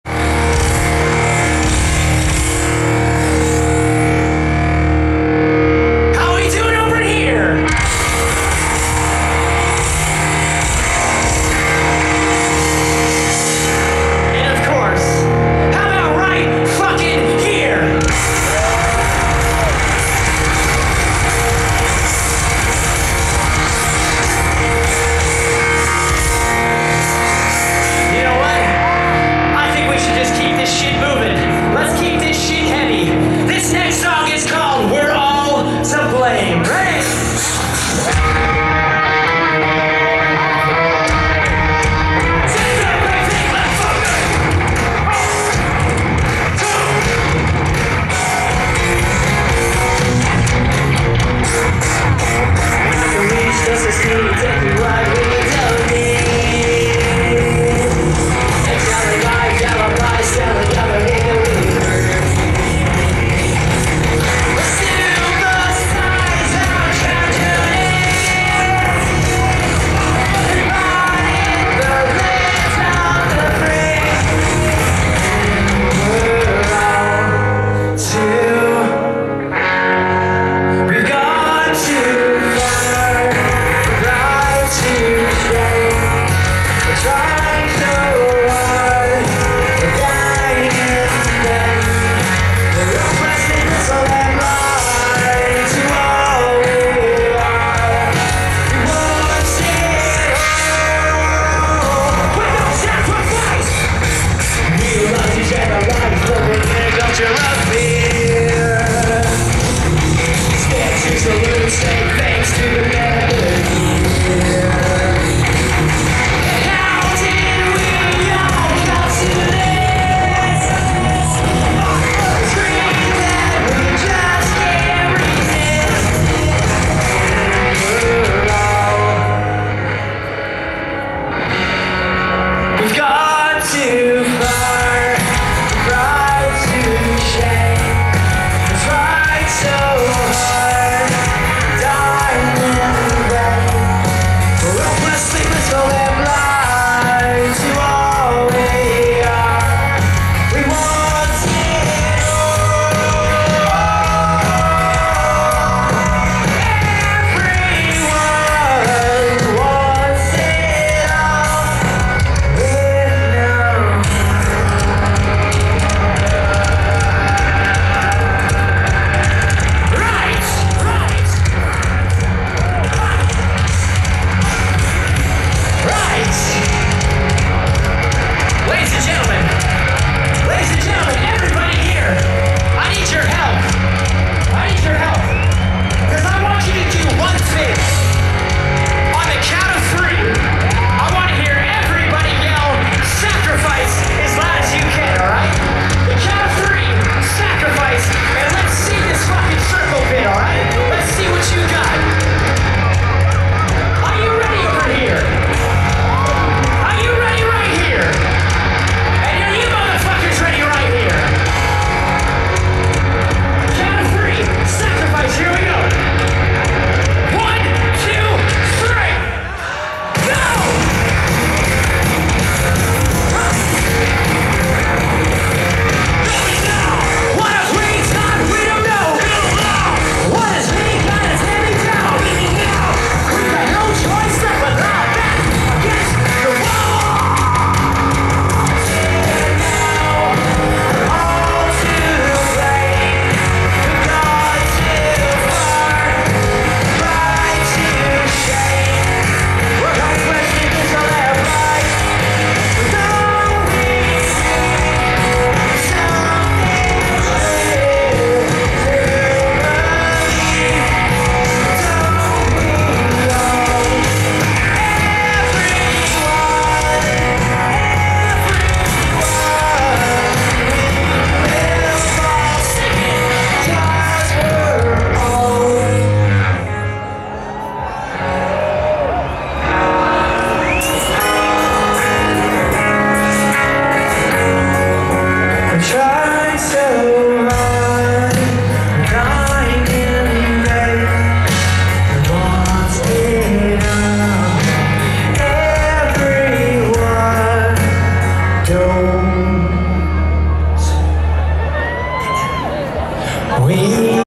SUM 41. Live in Céret 2022.
SUM 41 Live in Les deférlantes Fest 2022.
Céret. North Catalonia. France.